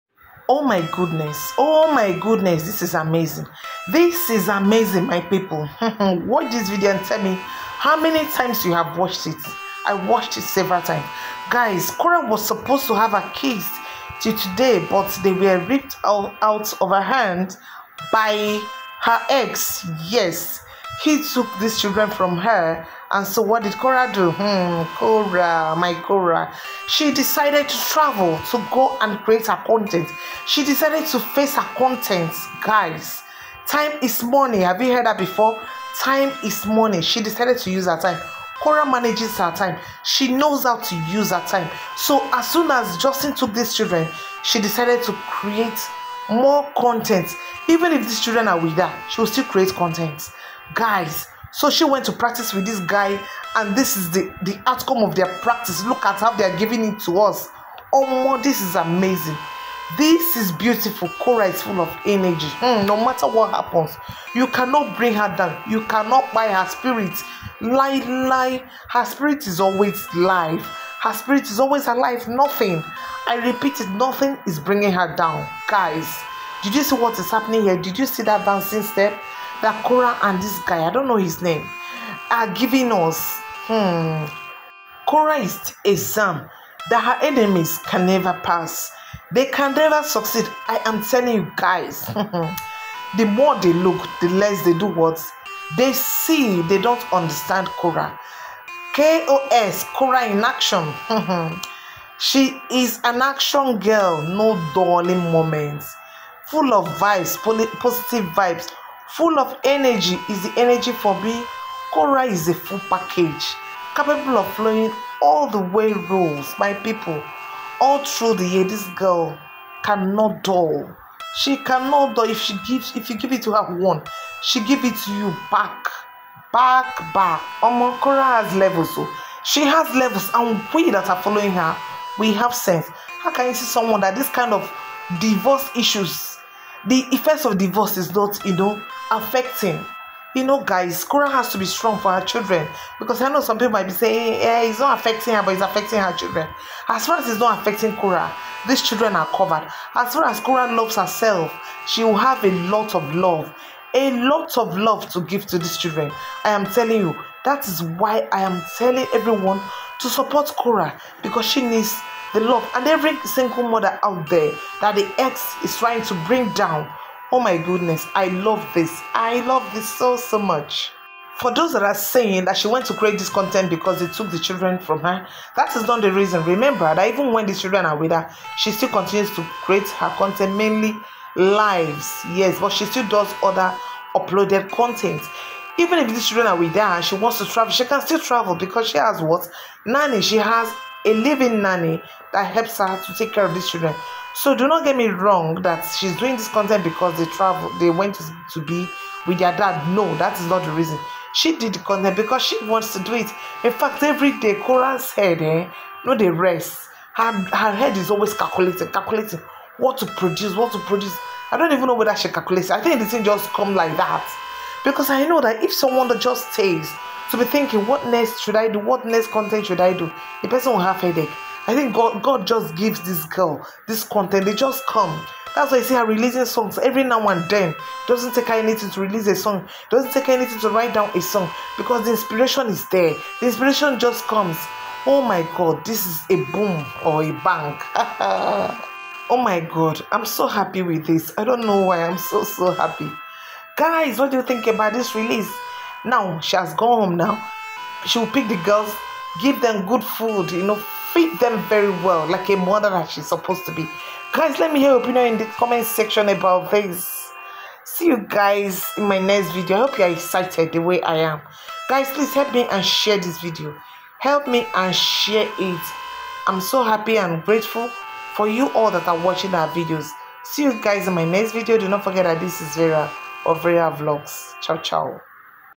Oh my goodness😱, Korra is full of energy, full of positive vibes. WOW 😲
Korra doing what she knows best to do after her ex has taken the children from her forcefully from June's school. Korra is full of energy. Full of vibes. Watch 👀👇